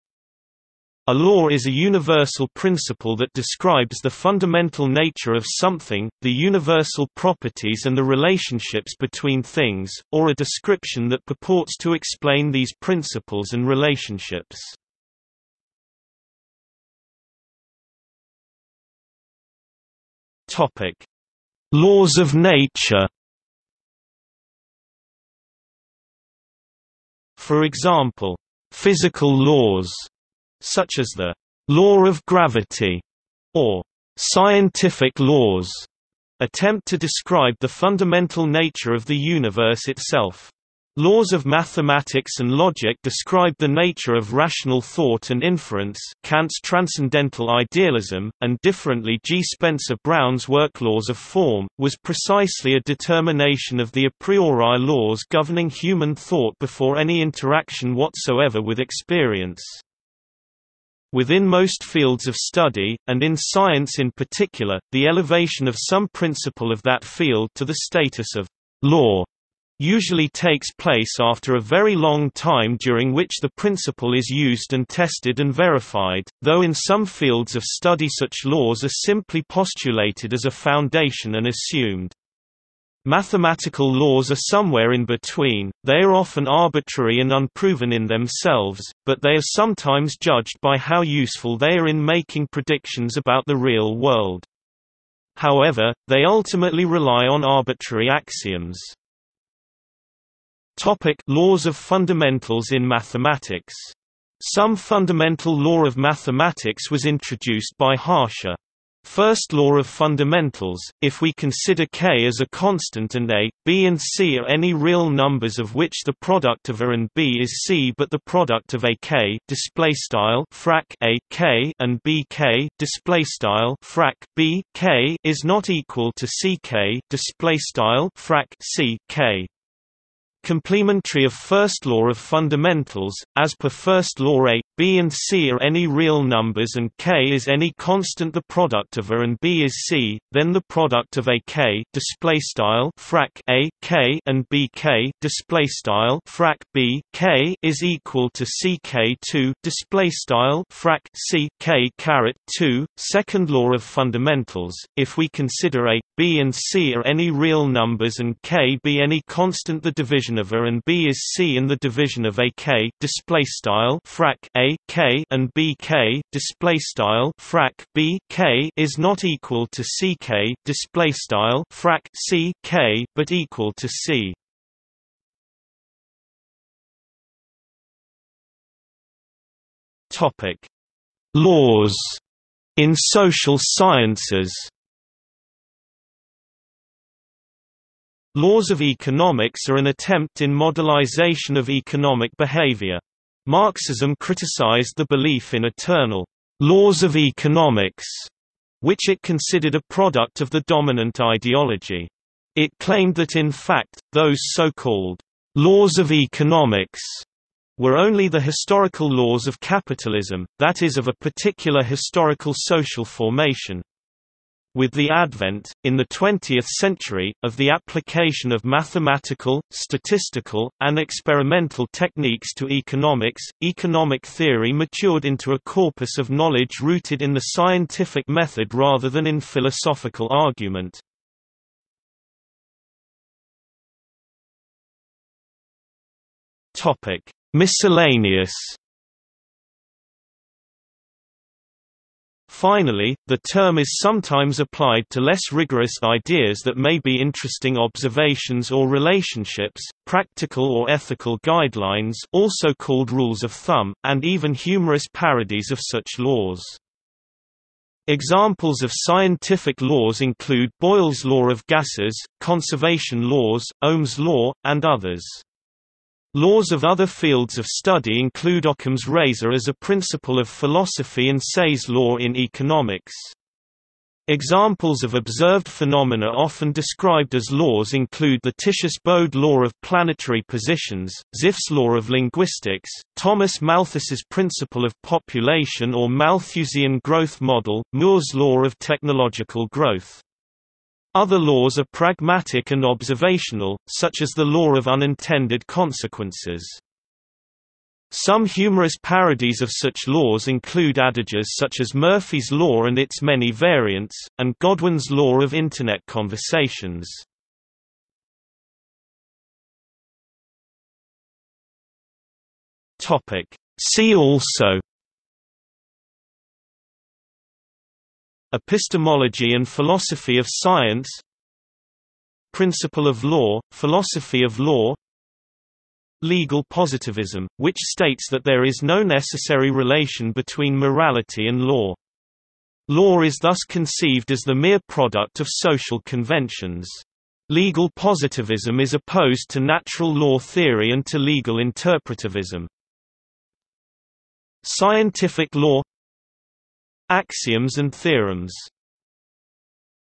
Law (principle) | Wikipedia audio article
00:00:16 1 "Laws of nature "
00:01:40 2 "Laws of fundamentals in mathematics"
00:02:56 3 "Laws" in social sciences
00:03:53 4 Miscellaneous
00:05:09 5 See also

- Socrates

SUMMARY
A law is a universal principle that describes the fundamental nature of something, the universal properties and the relationships between things, or a description that purports to explain these principles and relationships.